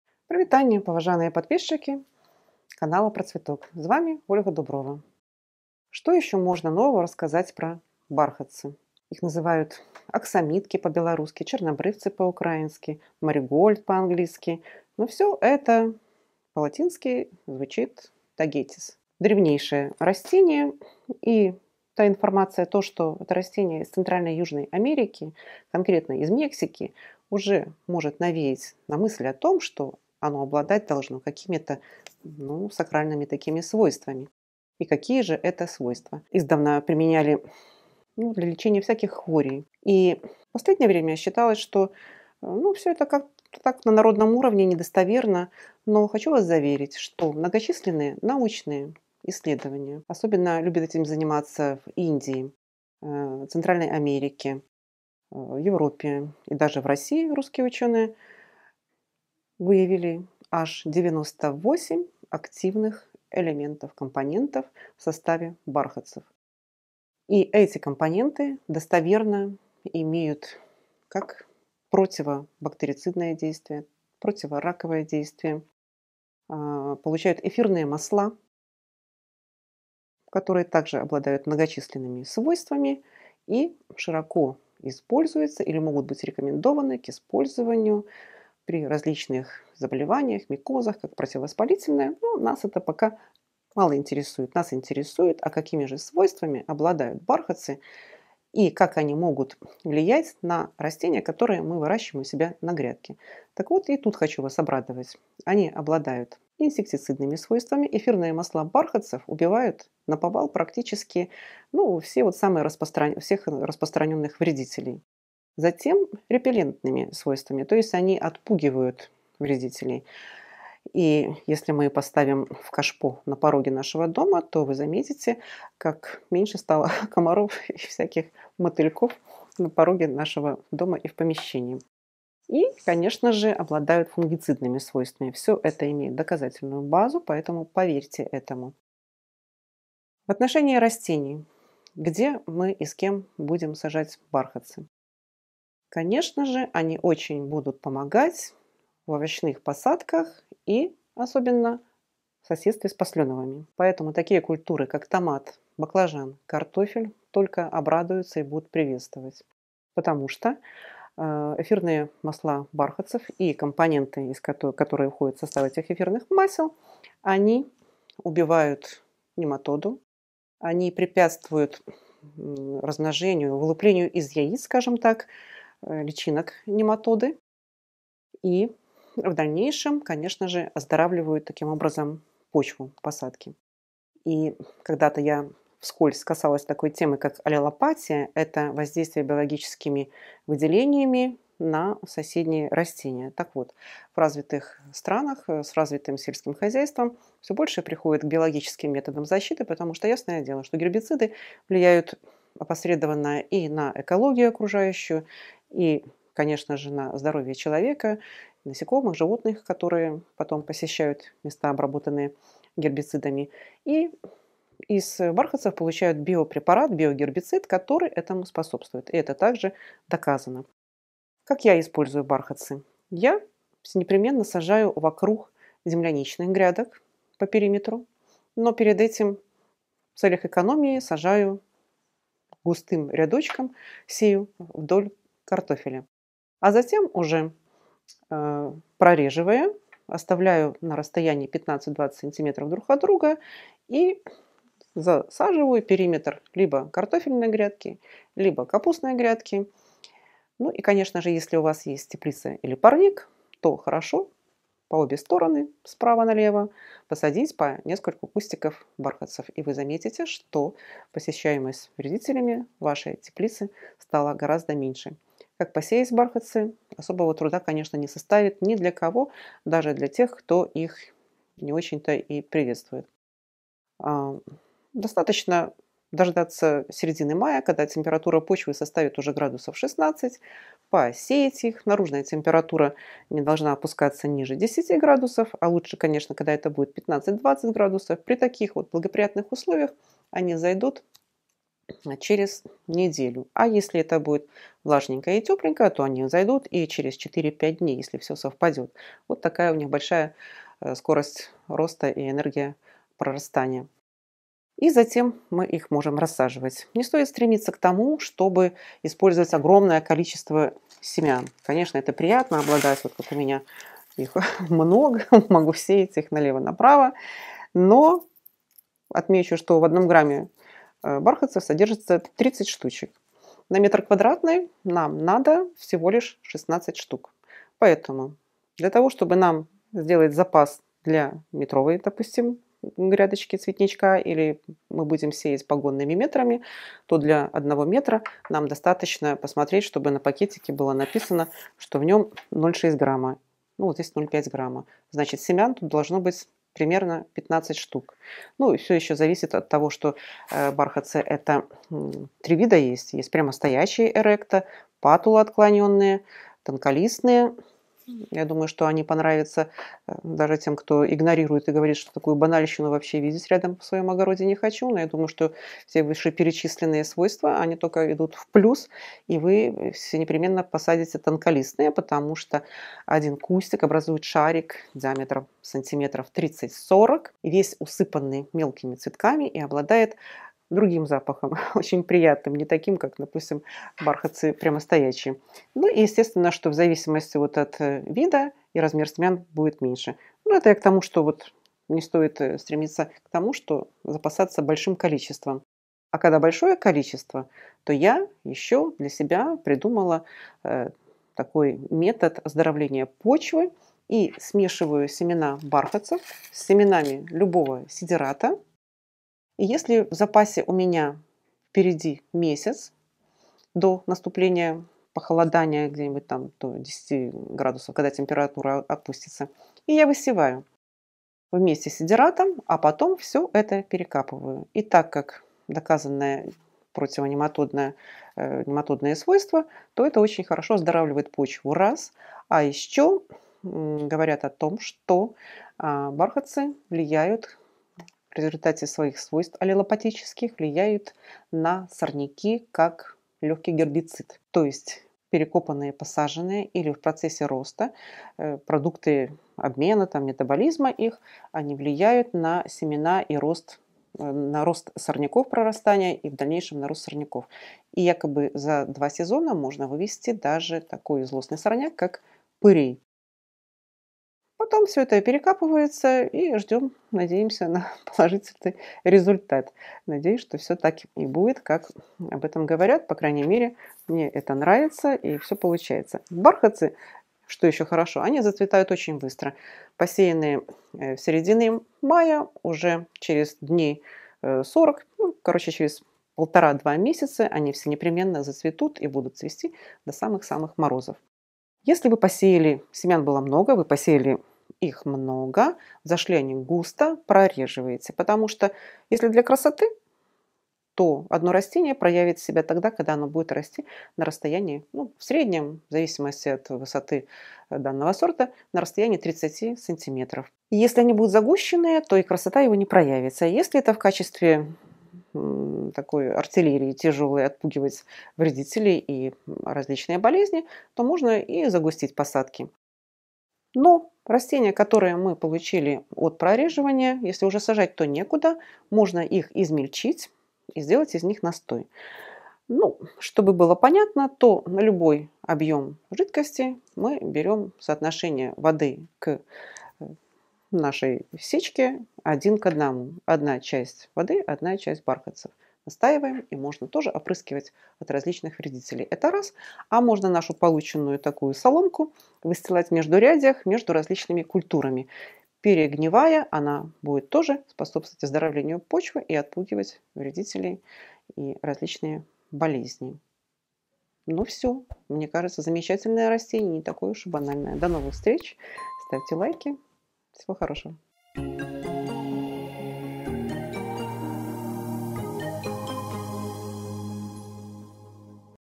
СЕЮ БАРХАТЦЫ ВЕЗДЕ И БЕД НЕ ЗНАЮ! Полезные свойства бархатцев в саду и огороде + хитрости посева
В этом видео научный сотрудник Ботанического сада Беларуси расскажет зачем она сажает бархатцы по всему участку. Бархатцы - уникальный цветок, который не только украсит ваш огород, но также оздоровит его и избавит от вредителей.

00:00 Вступление
00:19 Полезные свойства бархатцев
02:22 С какими растениями посадить бархатцы
04:25 Как посеять бархатцы
06:40 Оздоровление почвы бархатцами
08:13 Как густо сеять бархатцы